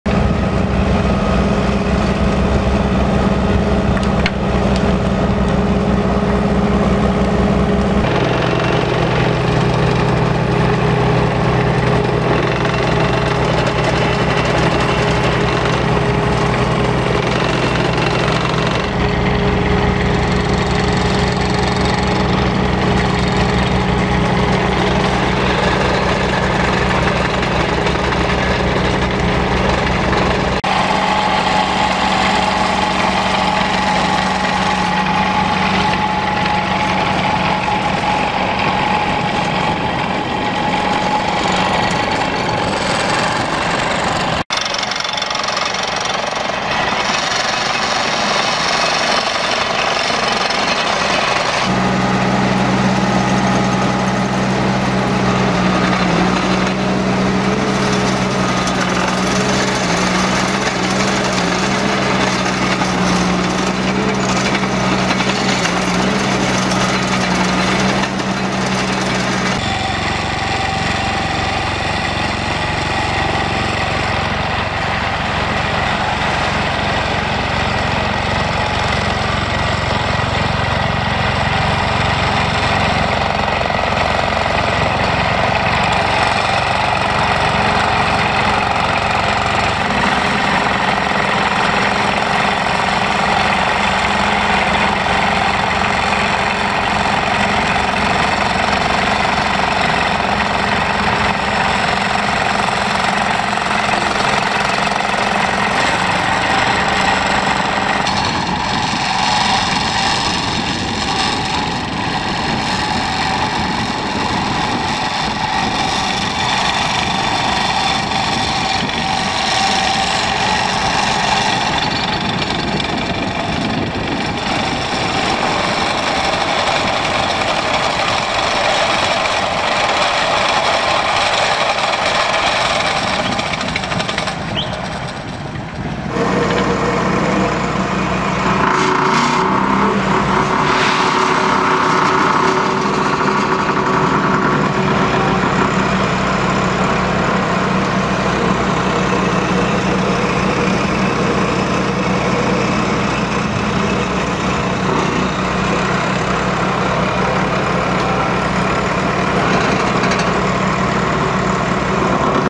Cable plough parallel to road - Kabelpflug parallel zur Straße
HUDDIG Kabelpflug arbeitet an einer Kreisstraße im Landkreis Uelzen um den Breitbandausbau mit LWL-Leerrohren voranzutreiben. Cable plough in Northern Germany mounted on a HUDDIG